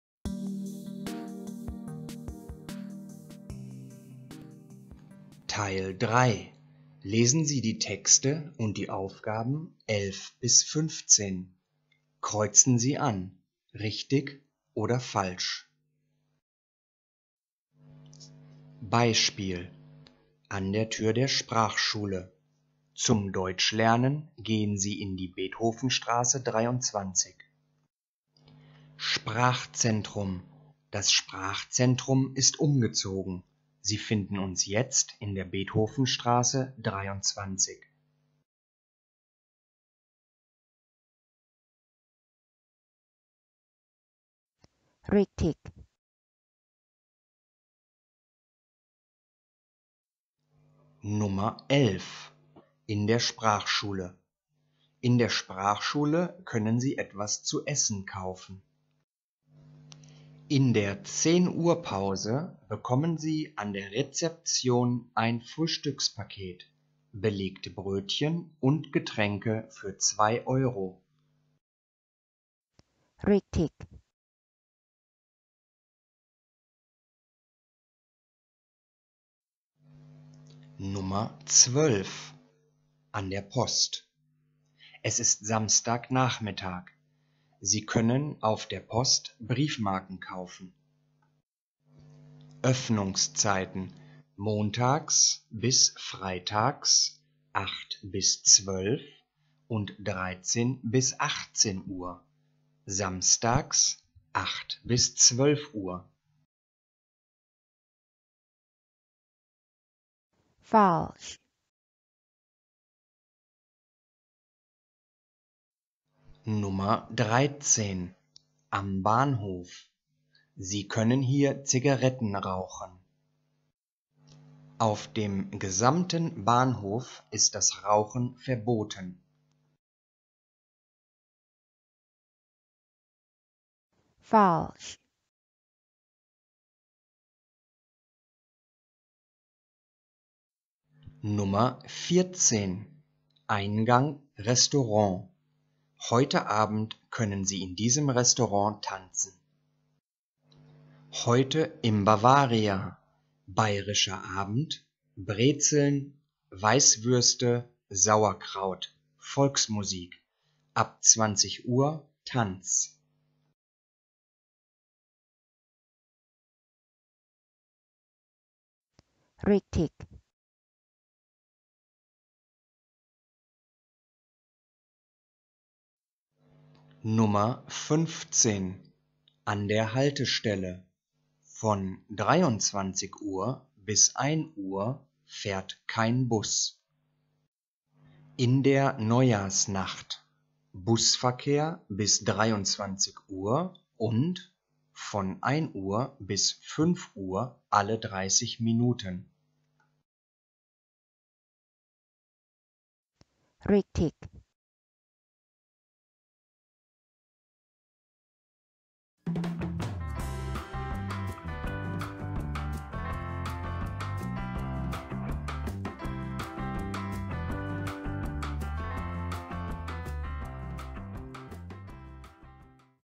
A1 Start Deutsch 1 Lesen Teil 3 Modellsatz
Start Deutsch A1 Lesen Teil 3 Modellsatz
German reading exam Part 3

Lesen Sie die Texte und die Aufgaben 11 bis 15.
Kreuzen Sie an: Richtig oder Falsch .